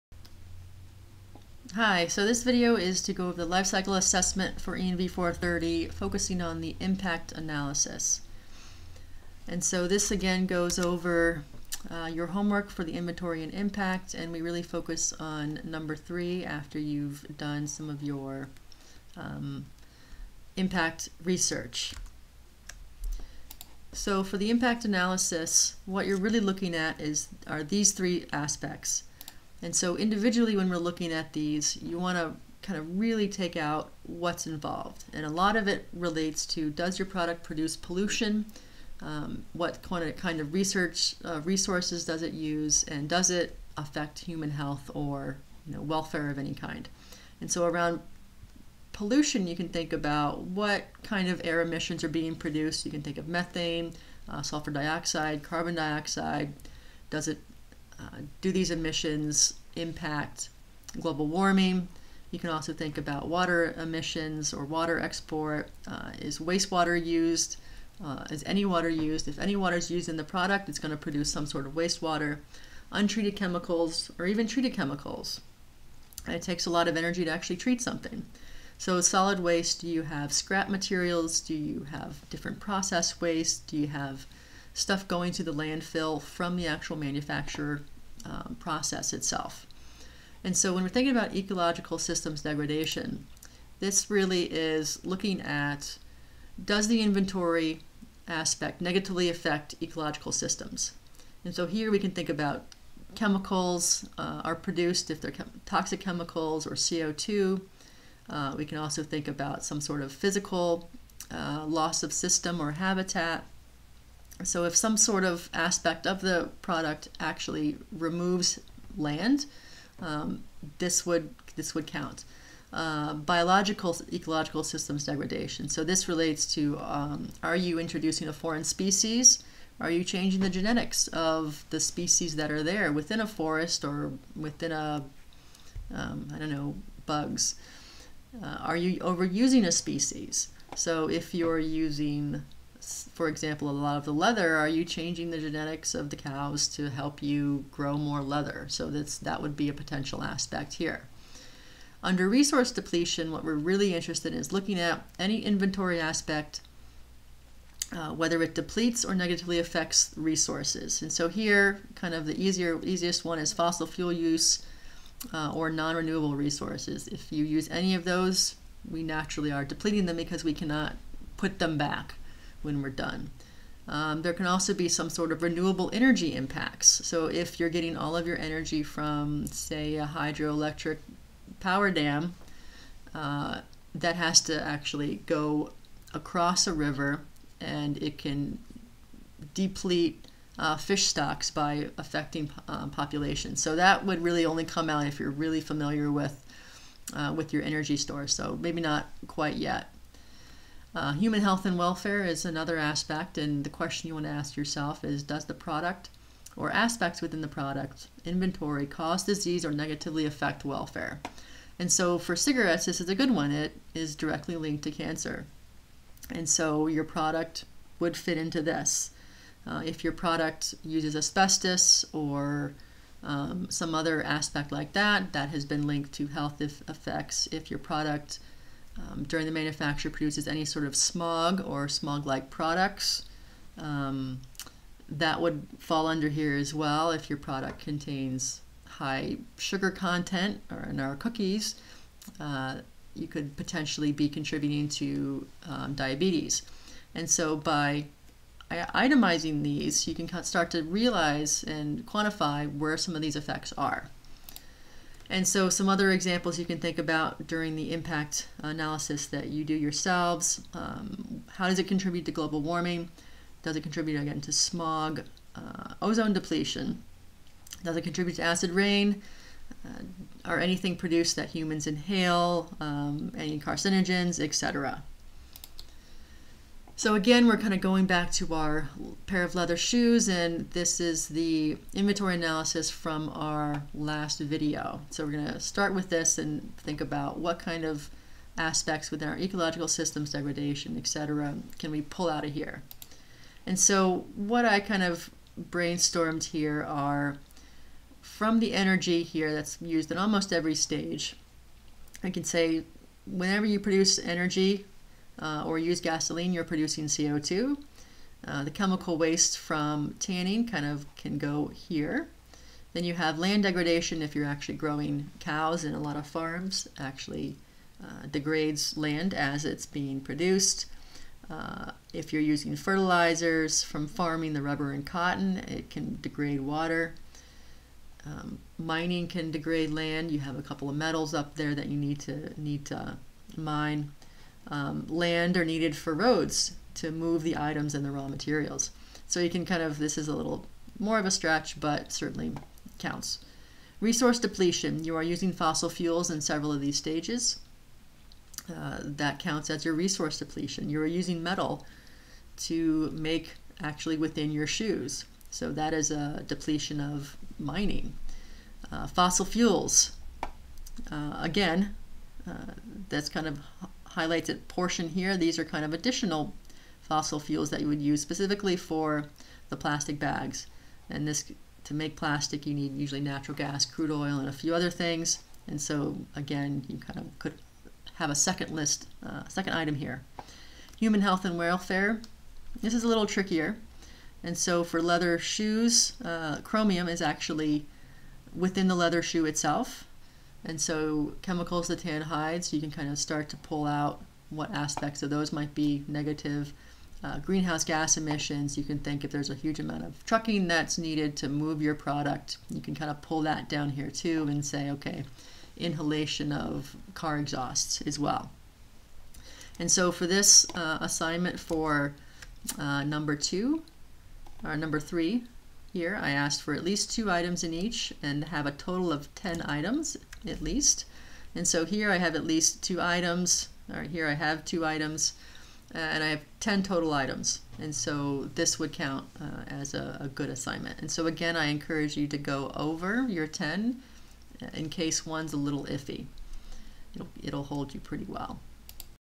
LCA Impact analysis
How to perform a LCA impact analysis for ENV430.